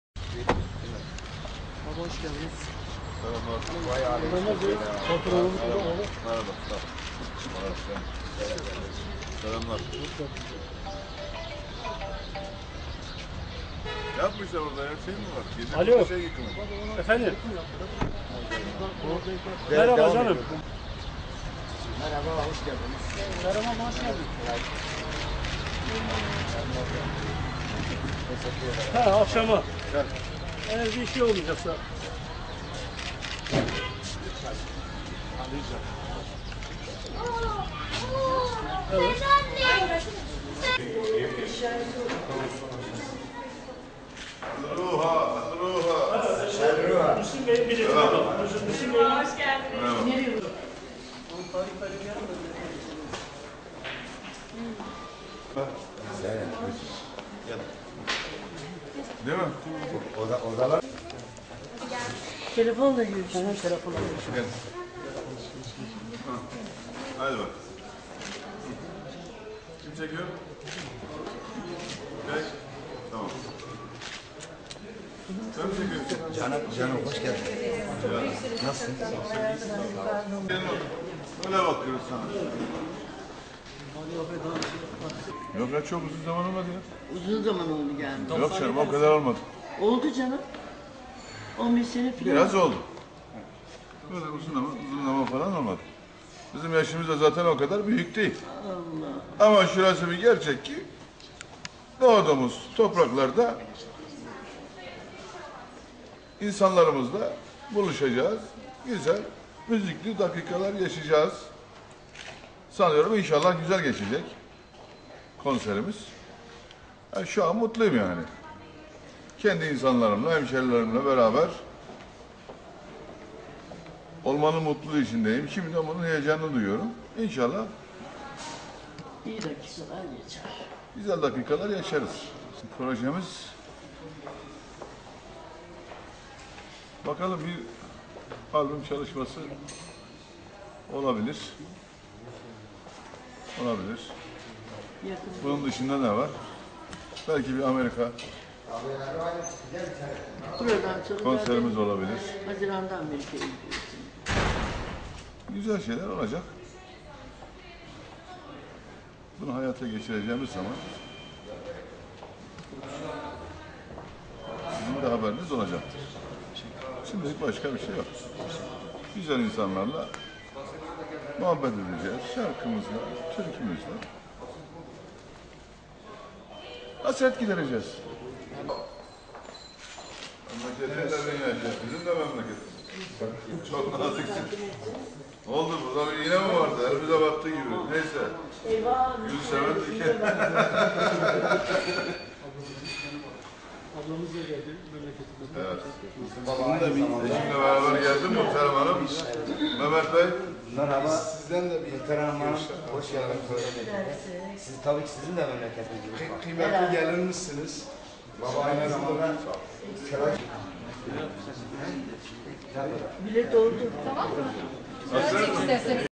Müslüm Baba açıklamalar
Şanlıurfa'da, konser vermek için uzun zamandan bu yana doğduğu kente gelemeyen şarkıcı Müslüm Gürses, hayranları tarafından çiçekle karşılandı. Şanlıurfa'da bulunmaktan dolayı çok mutlu olduğunu belirten Gürses, önümüzdeki günlerde Amerika da konser vereceğini söyledi.

Konser vermek için hayat arkadaşı Muhterem Nur ile birlikte karayoluyla Şanlıurfa'ya gelen arabesk sanatçısı Müslüm Gürses hayranları tarafından çiçeklerle karşılandı. Spor giysiler içerisinde konakladığı otele geçen Gürses, sık sık hayranları tarafından yolu kesilerek hatıra fotoğrafı çektirdi.
Siyah güneş gözlüklerini otel lobisinde bile hiç çıkartmayan Müslüm Gürses, gazetecilere açıklamada bulundu.

Uzun yıllardan bu yana Şanlıurfa'ya gelemediğini belirten Müslüm Gürses, şöyle dedi:
"Bugün doğduğum topraklarda insanlarımızla buluşup onlarla müzikli dakikalar yaşayacağız. Bu yüzden hemşerilerimle hasret gidereceğim için çok mutluyum. Önümüzdeki aylarda yeni albüm çalışmam olacak. Yine Amerika'da bir konser vermeyi düşünüyoruz."
Bir süre konser organizatörleriyle görüşen ünlü sanatçı daha sonra dinlenmek üzere otel odasına çekildi.